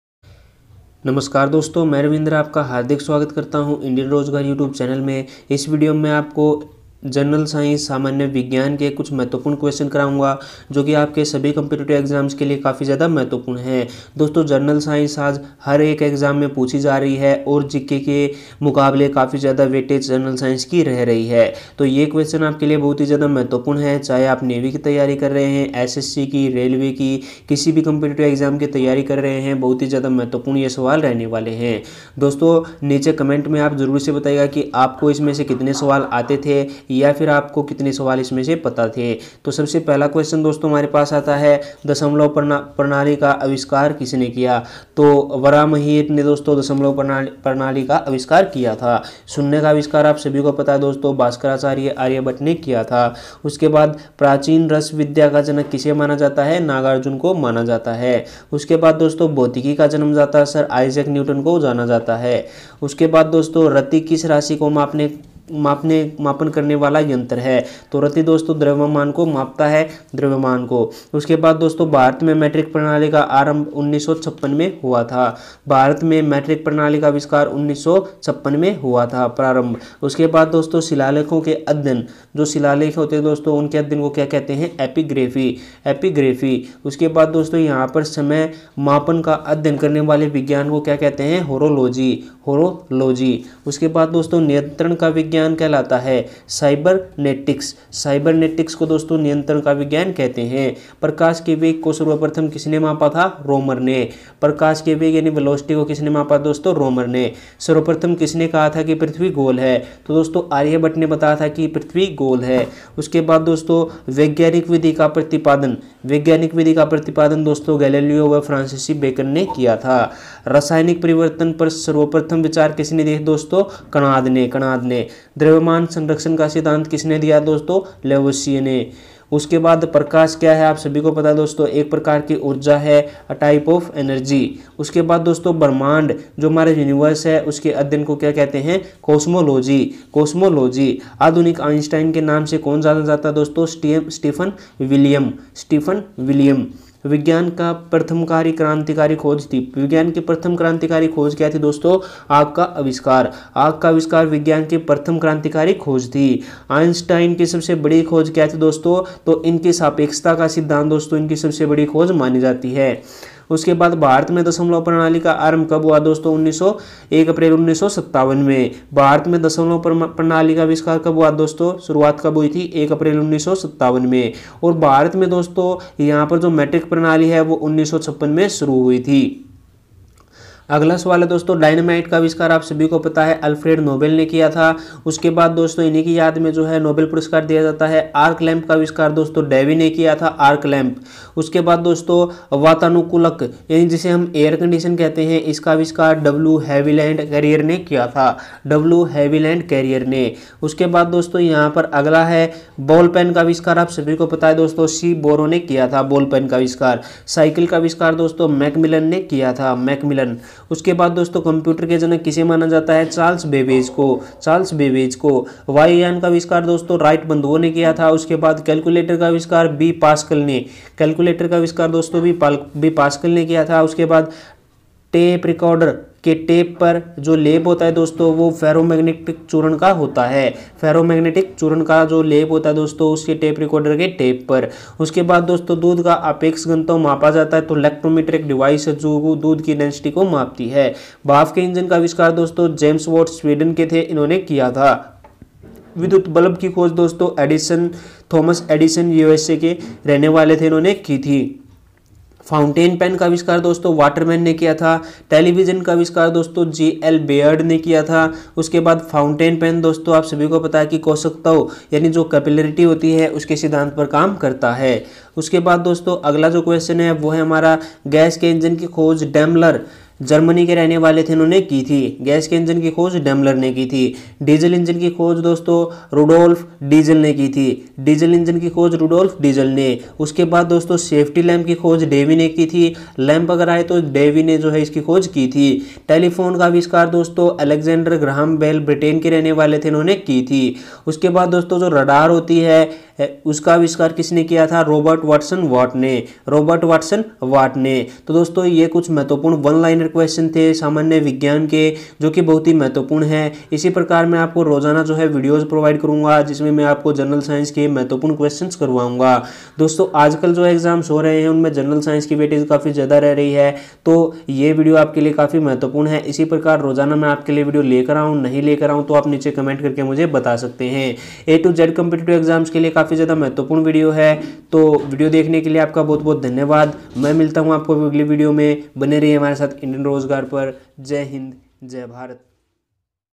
GS Important One Liner Questions Answer | SSC | Railway | Navy Exams
Welcome To Indian Rojgar

In this video I teach you important general science question for all competitive exams like ssc,railway and navy. Please watch video till end.

Complete GK PDF Book For Navy MR/SSR/AA,Airforce Y group ,Coast Guard